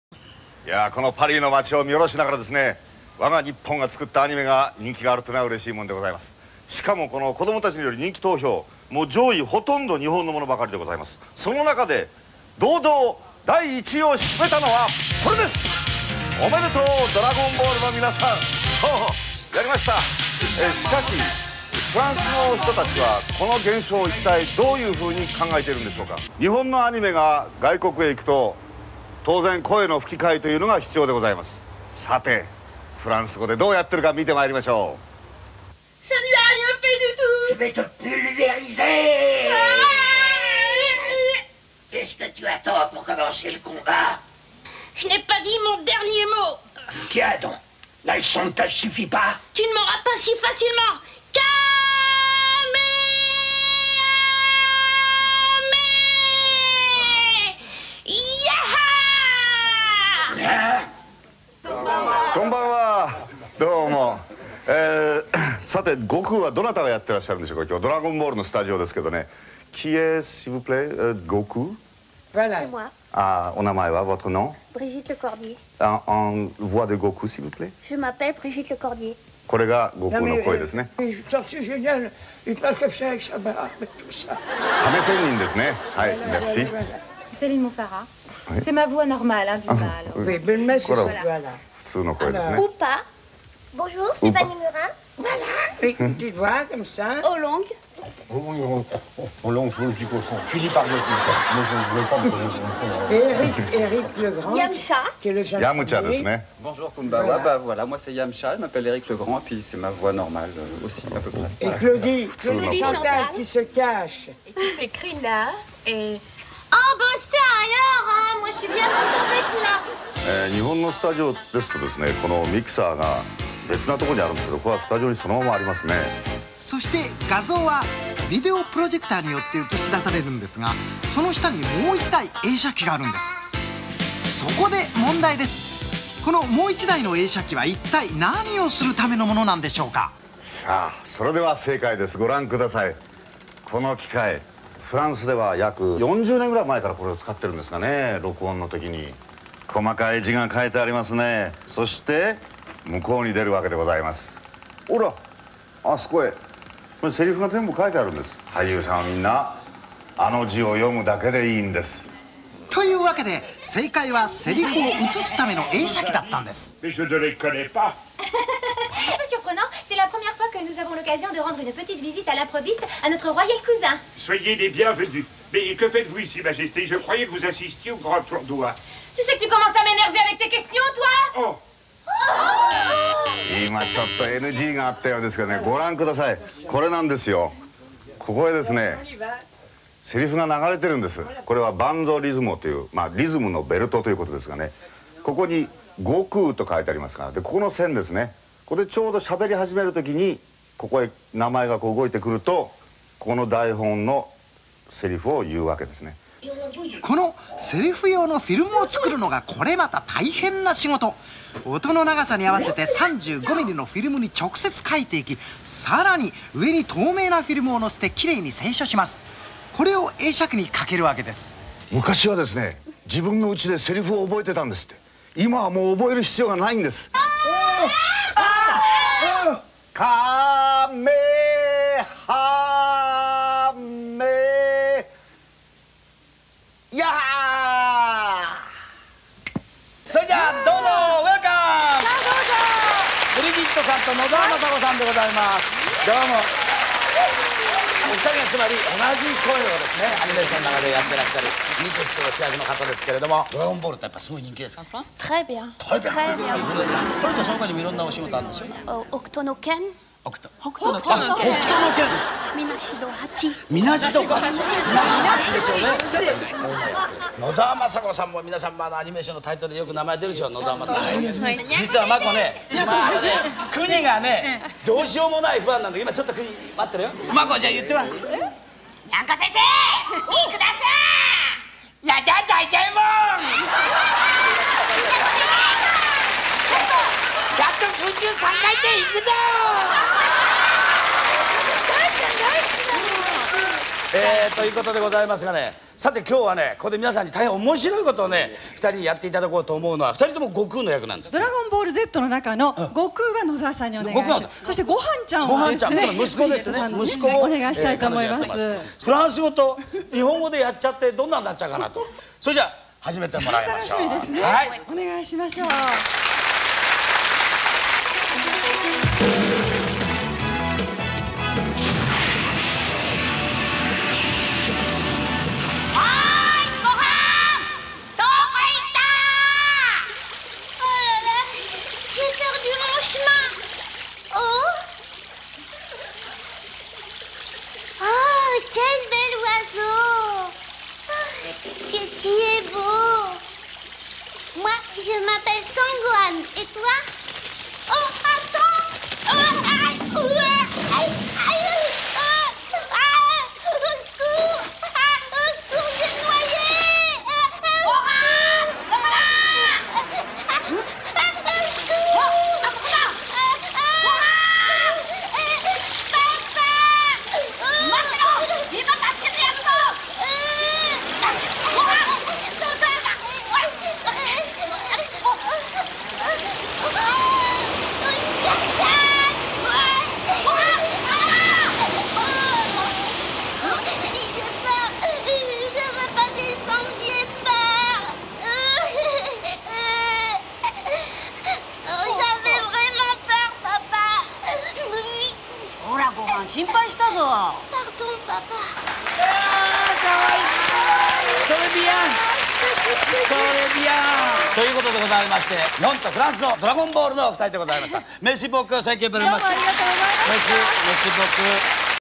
INÉDIT ! Dragon Ball - Visite Studios AB + Inédit Duo Nozawa + Lecordier !
Une superbe vidéo des jap qui visitent les studios AB dans les années 90, avec en prime un duo Mamie Nozawa feat. Brigitte Lecordier ! Le présentateur doit venir du Mexique je pense :)

Thanks for the original uploader katobrucelee08, i upload for fun and for the purpose that it wont be lost ! I love it !